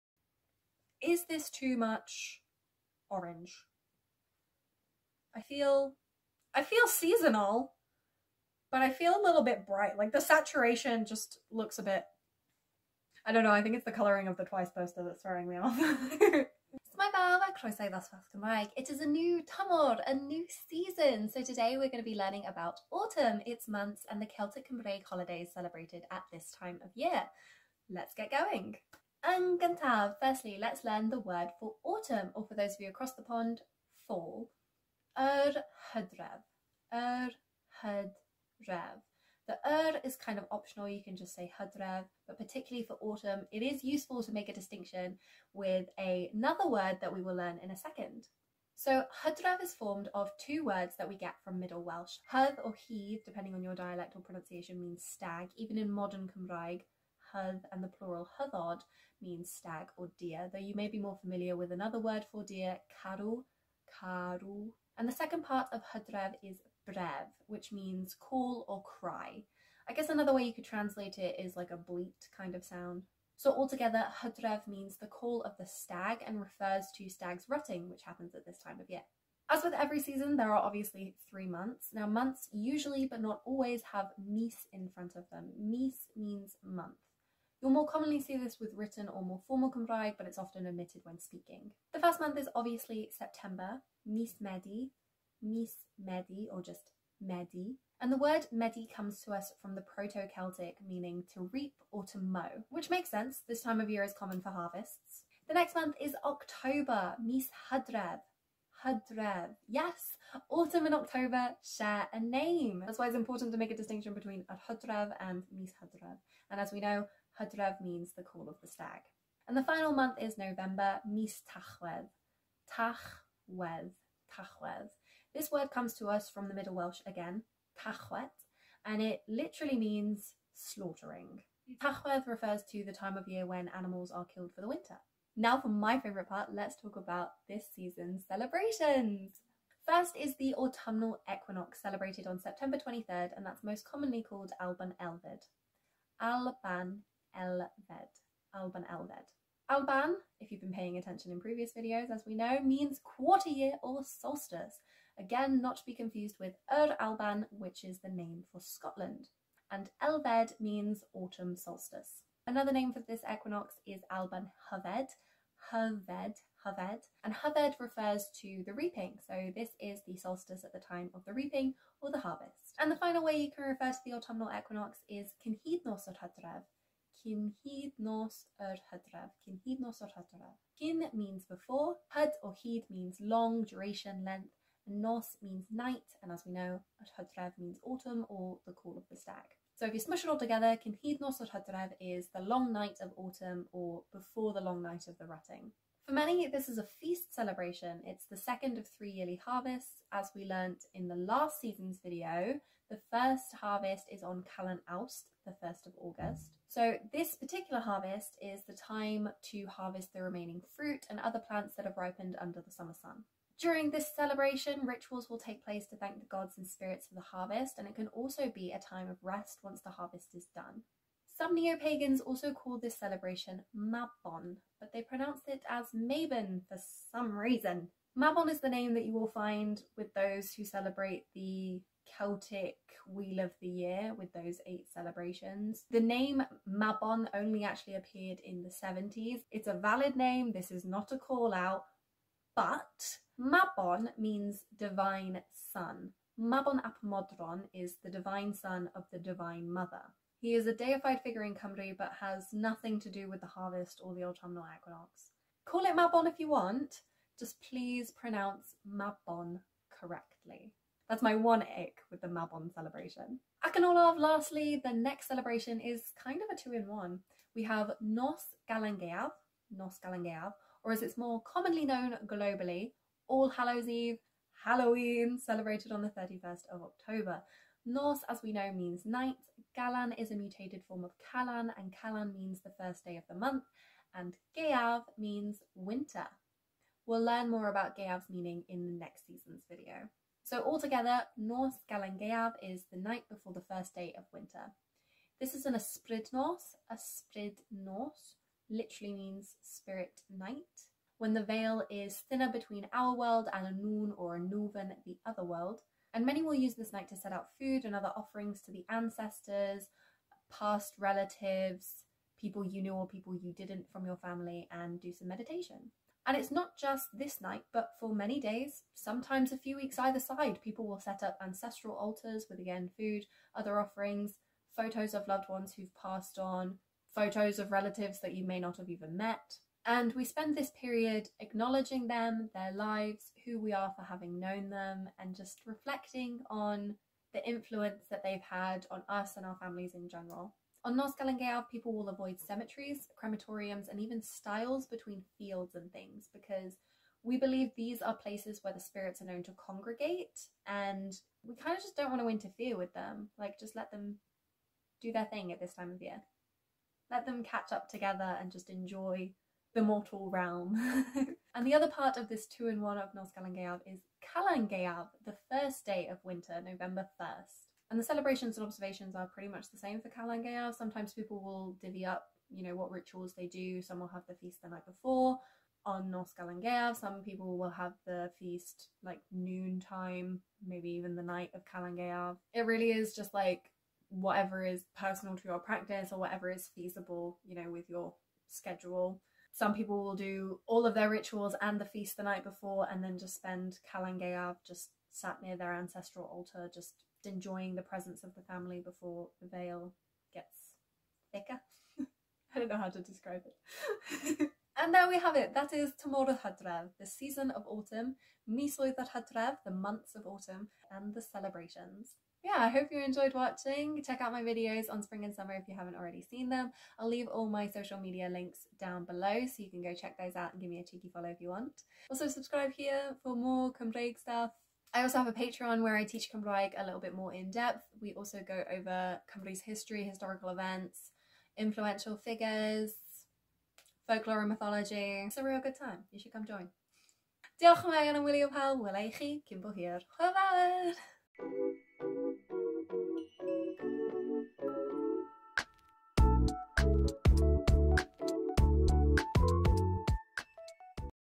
Yr Hydref / The Autumn | Dosbarth Cymraeg
S'mae!

Let's learn about autumn in Welsh / yr hydref yn Gymraeg